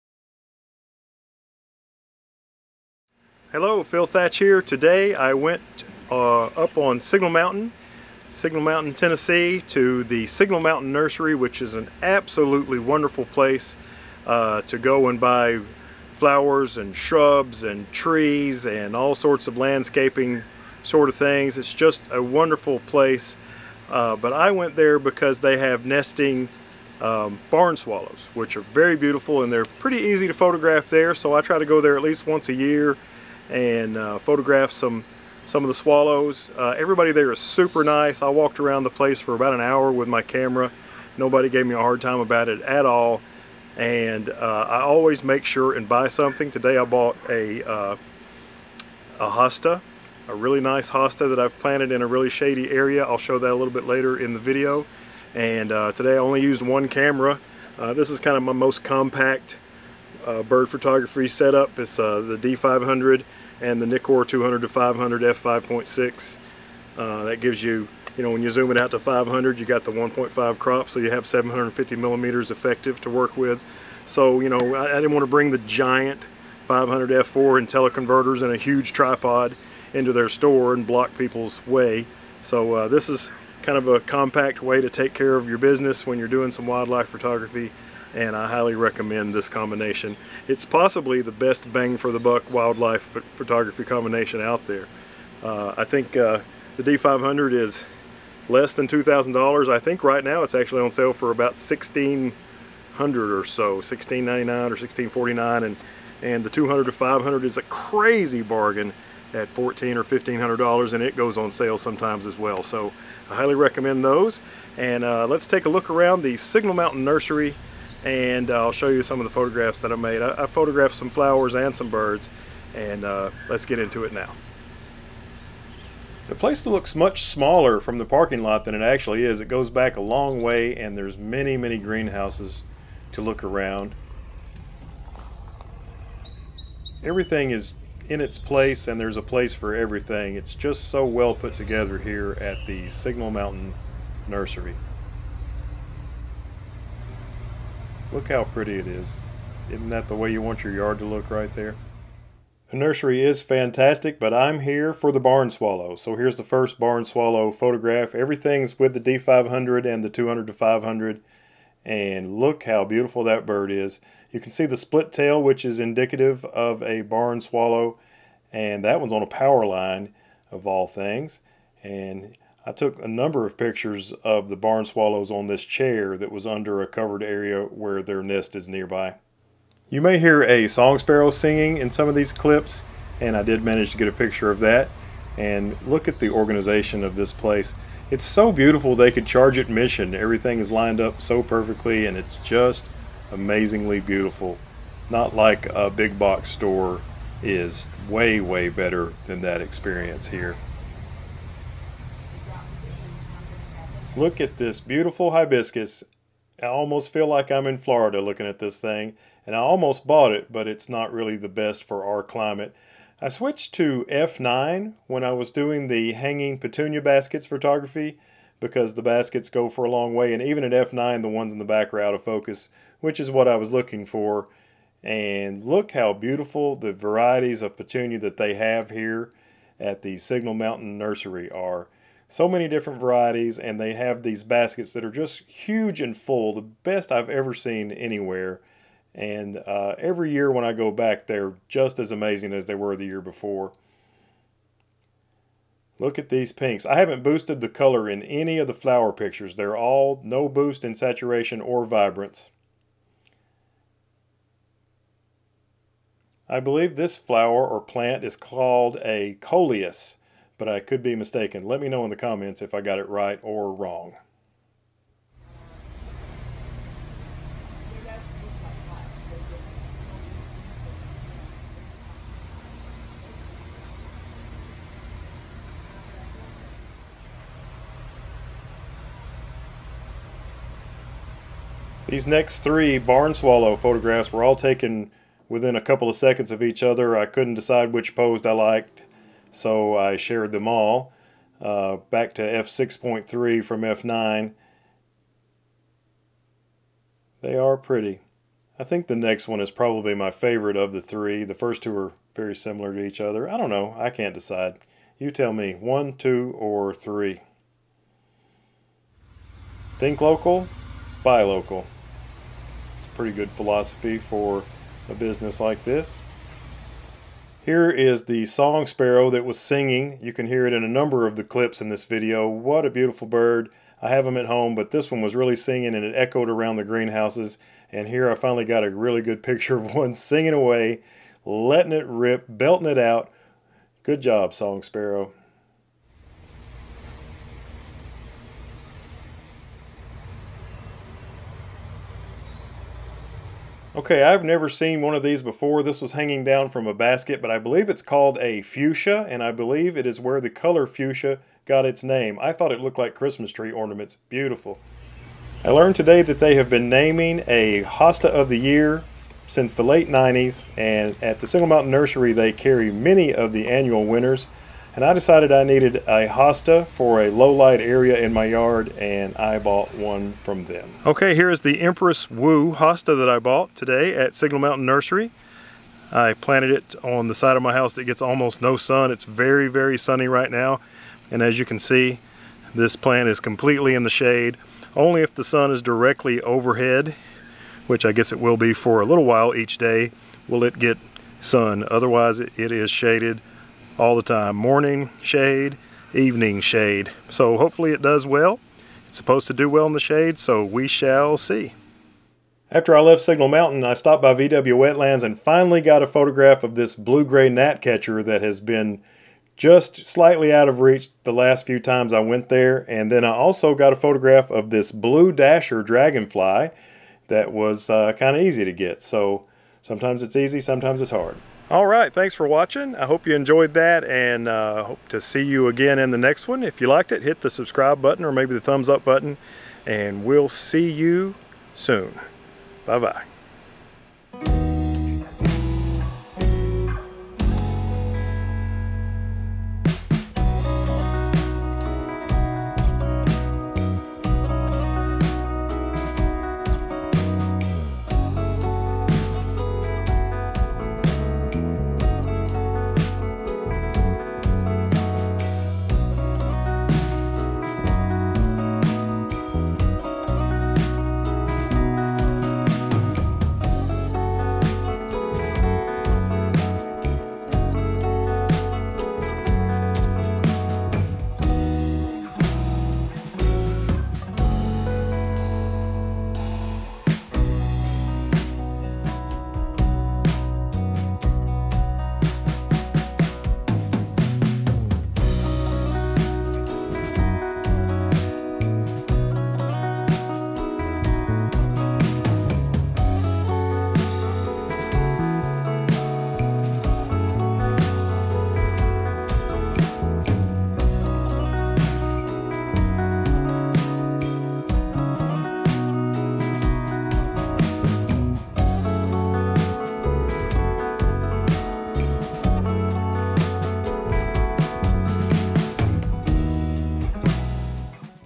Nikon D500 & 200-500 Flower and Barn Swallow Bird Photography
I went to Signal Mountain Nursery with my Nikon D500 and the Nikkor 200-500 f/5.6 zoom lens to photograph Barn Swallows and the beautiful flowers they have there. I came home with some nice images and a hosta that should be hardy for years to come.